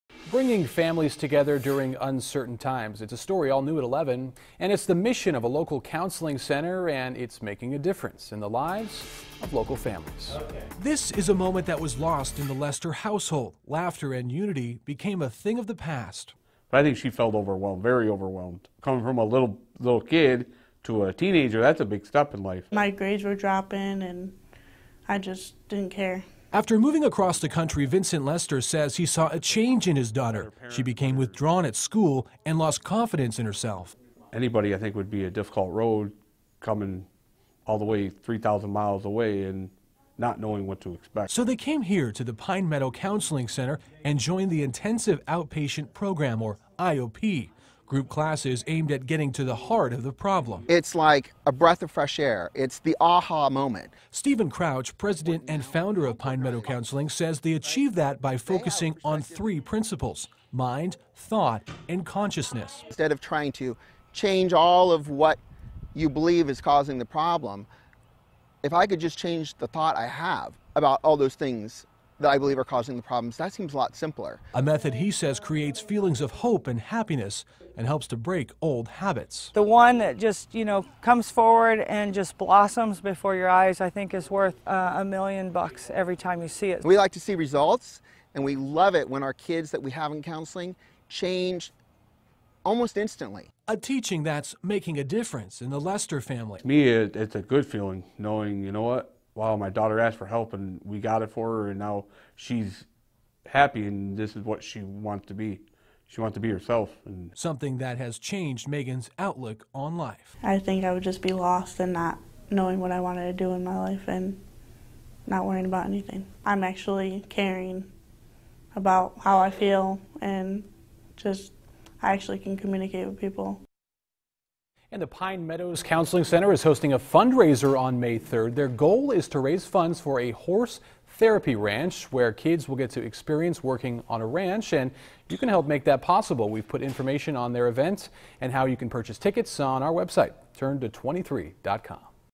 Local counseling center helping families in need
Using three principles and group classes.